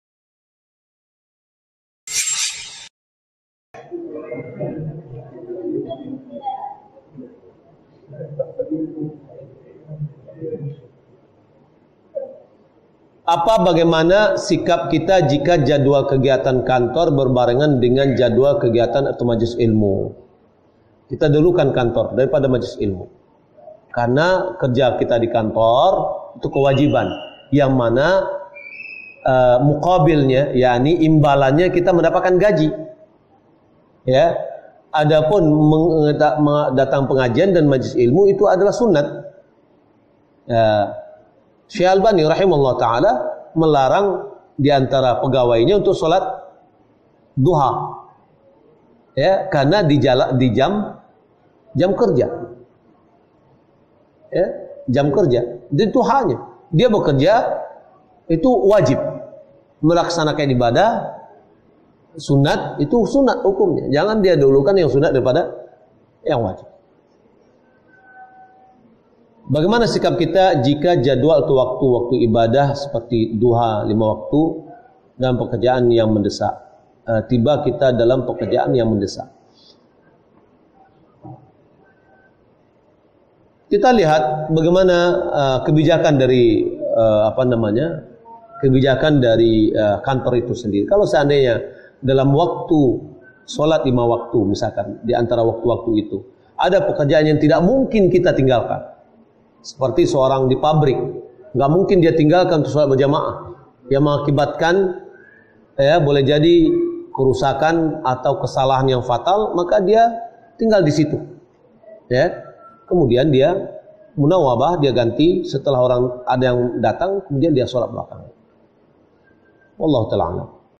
Bagaimana Sikap Kita Jika Jadwal Kajian Benturan Dengan Jam Kerja ? - Buya M Elvi Syam, Lc, MA
Bagaimana Sikap Kita Jika Jadwal Kajian Benturan Dengan Jam Kerja ?
Oleh : Buya M Elvi Syam, Lc, MA
Sahabat, bila video ini bermanfaat,
Share, yuk! Semoga saudara² kita mendapatkan faidah ilmu.